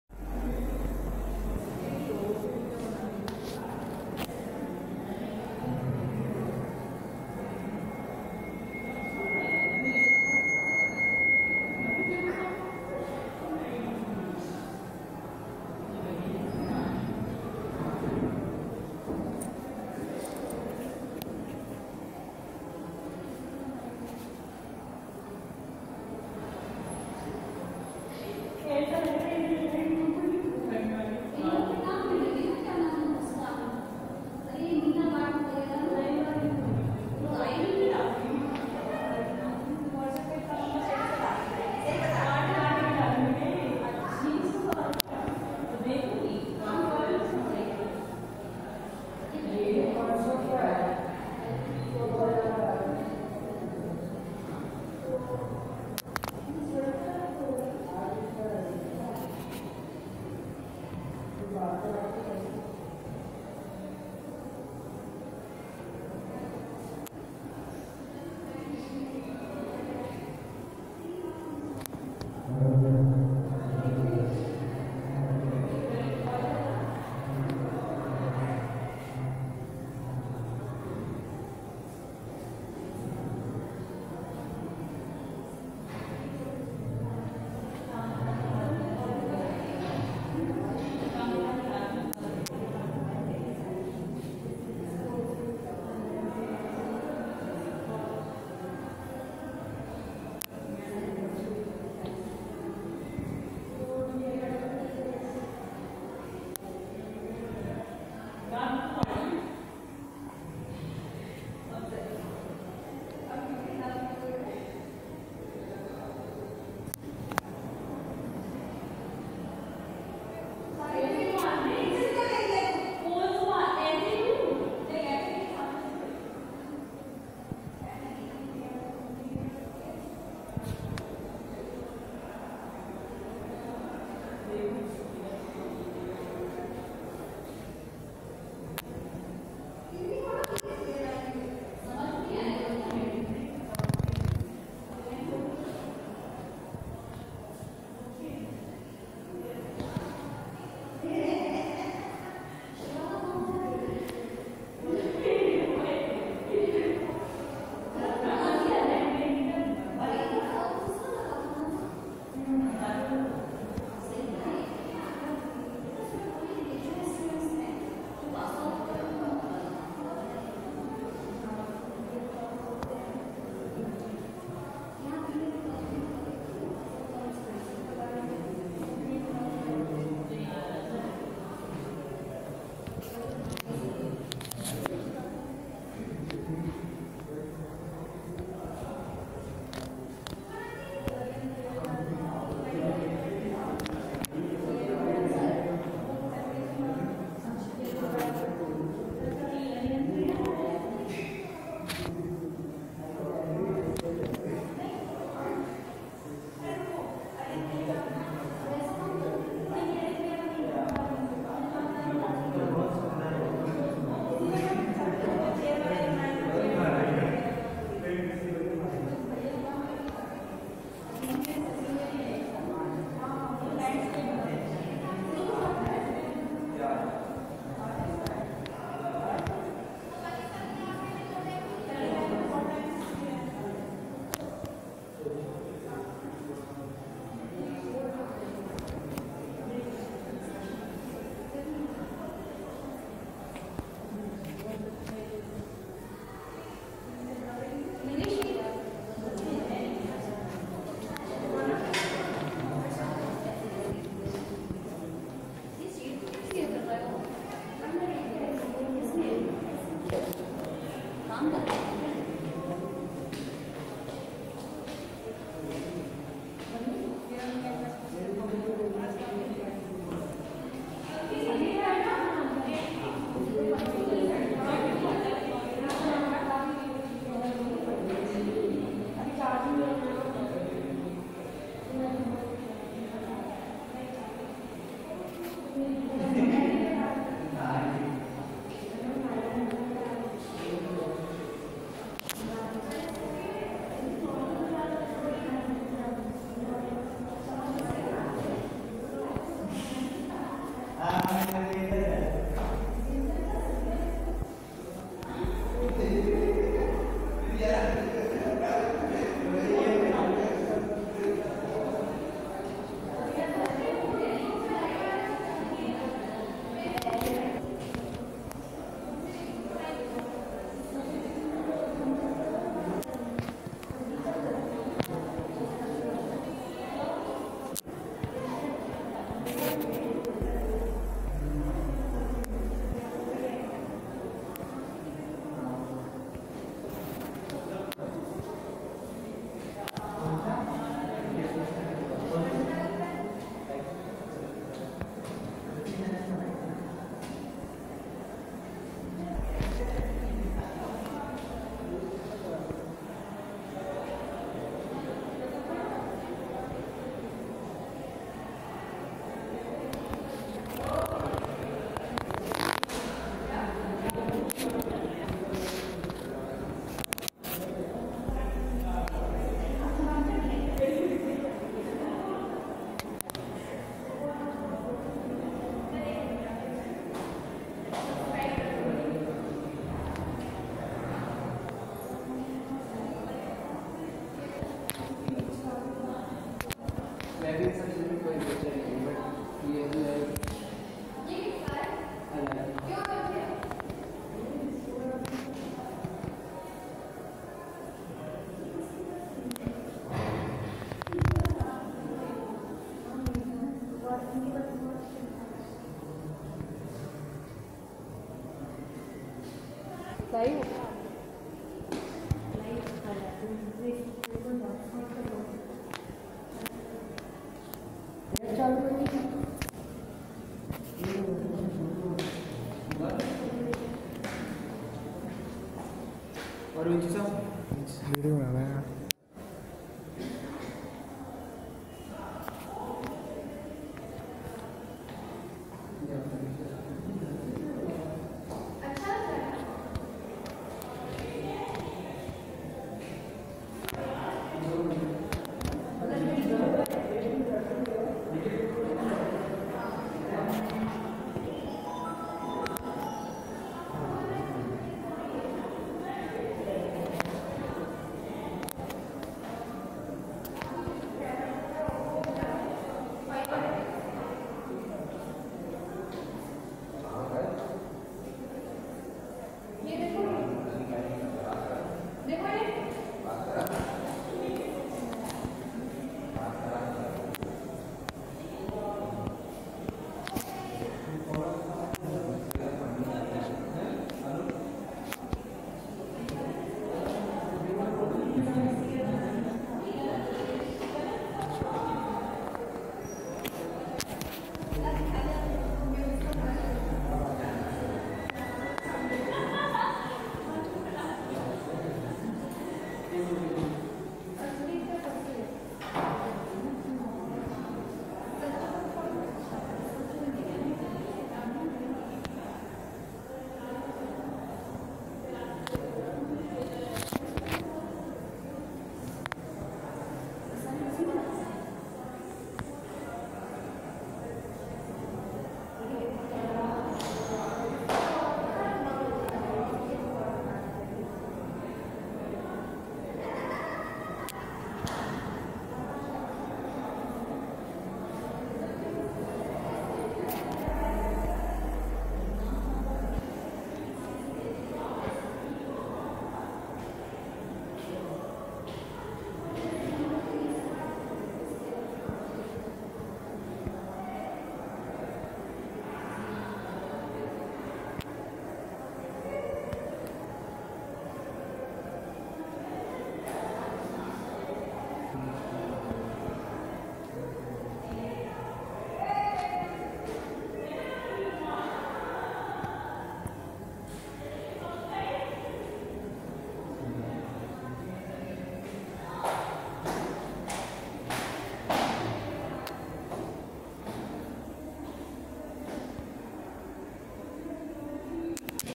Painting Exhibition at Raja Man Singh Tomar Music & Art University, Gwalior
Celebrating Basant Panchmi 29/01/2020